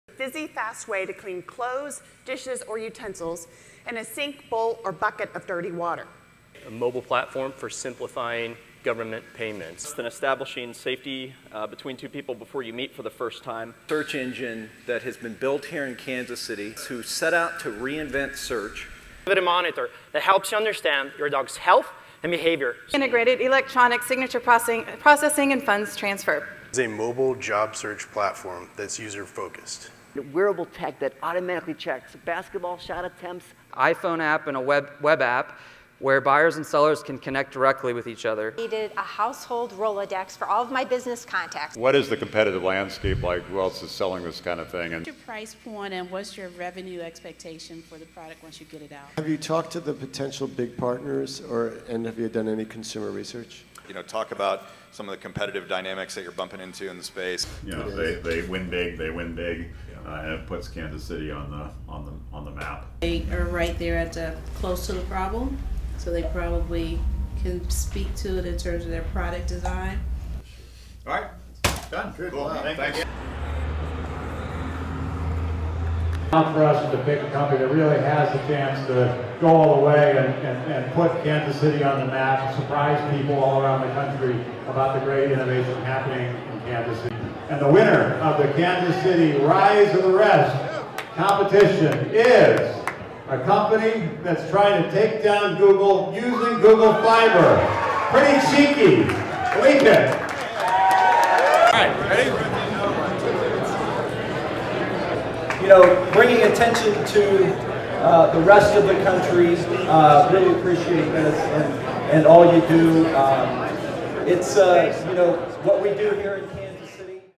Rise of the Rest Road Trip: Kansas City Pitch Competition.
See the participants and winner of the Rise of the Rest pitch competition in Kansas City.